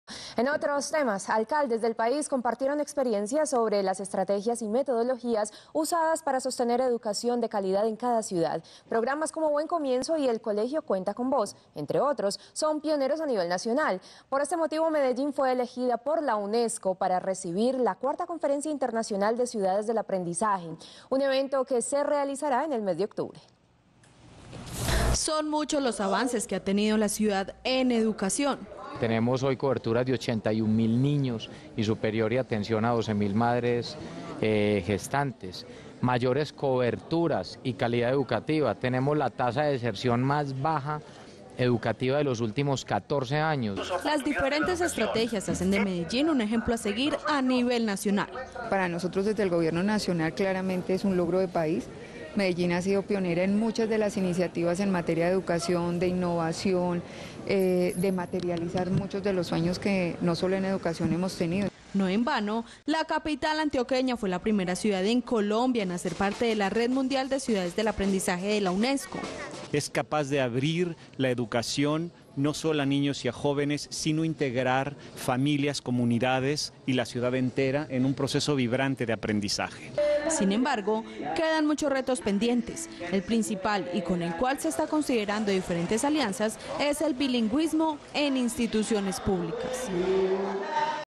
Alcaldes del país hablan de estrategias para sostener educación de calidad
Alcaldes del país compartieron experiencias sobre las estrategias y metodologías usadas para sostener educación de calidad en cada ciudad.
Programas como Buen Comienzo y el colegio cuenta con vos entre otros, son pioneros a nivel nacional. Por este motivo Medellín fue elegida por la Unesco  para recibir la conferencia internacional de ciudades del aprendizaje, evento que se realizara en el mes de octubre.